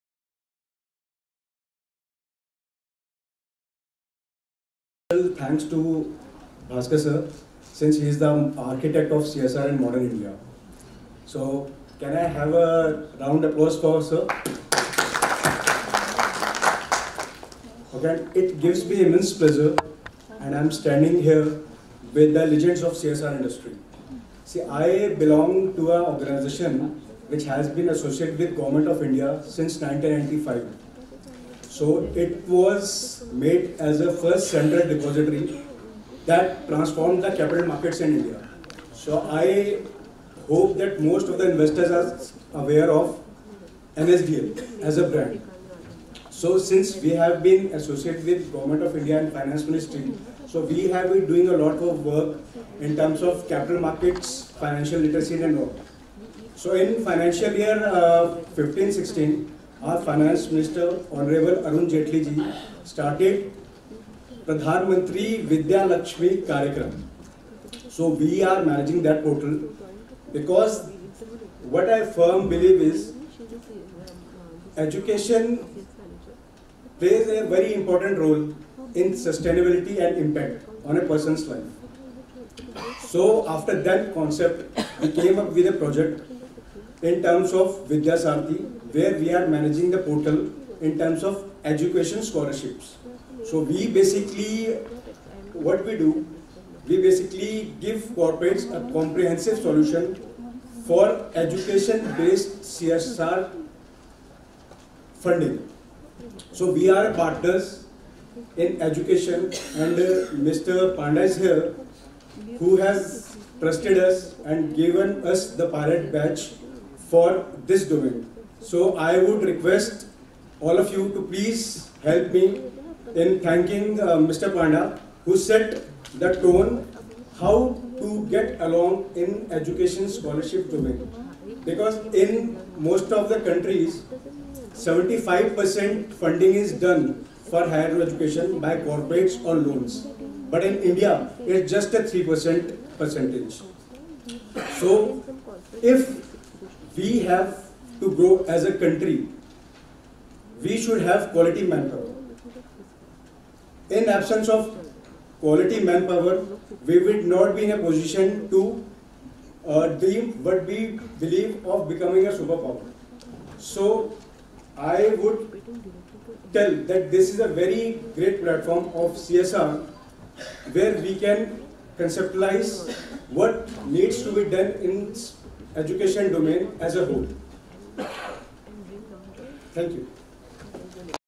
DHEERAJ LATH AT GIVING BACK INDIA CSR LEADERSHIP SUMMIT 2019
ndia CSR, India’s largest CSR Network organized mega annual forum Giving Back India CSR Leadership Summit 2019 in New Delhi

on 1 April first at PHD House. Esteemed Guest of the Summit Shri Dheeraj Lath, Manager - NSDL E-Governance Infrastructure Ltd.